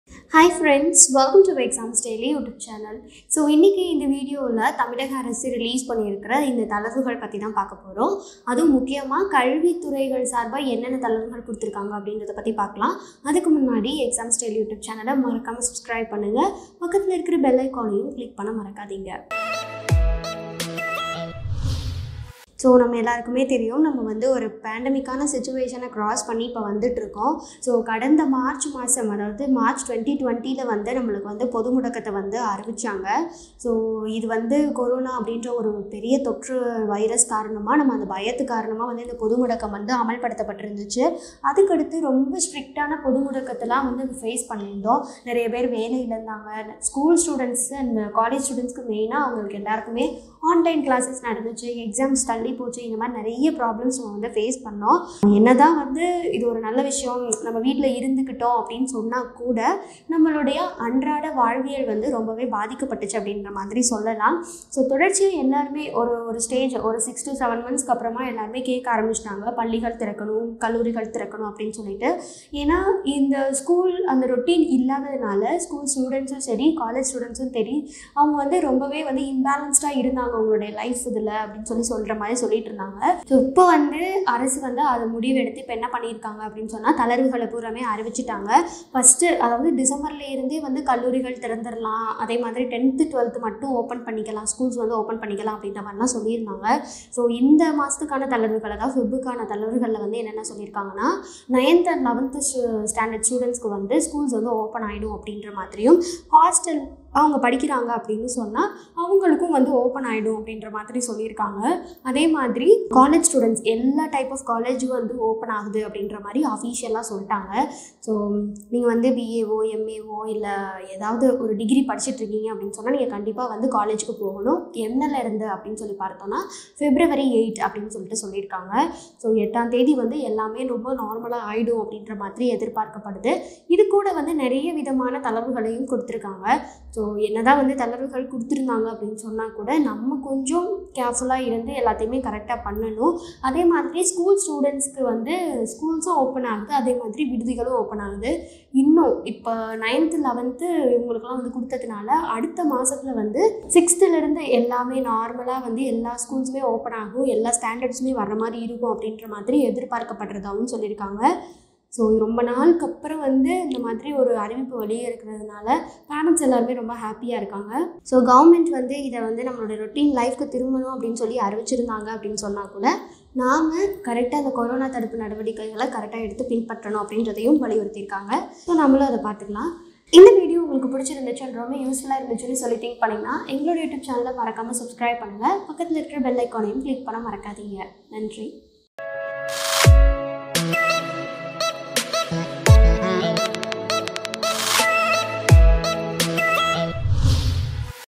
Feb 8 - தமிழகத்தில் பள்ளி, கல்லூரிகள் திறக்க அனுமதி ! | School College Reopen News in Tamilnadu
Tamilnadu Government has approved the permit for school and college reopen and it is the latest and important news today in Tamilnadu.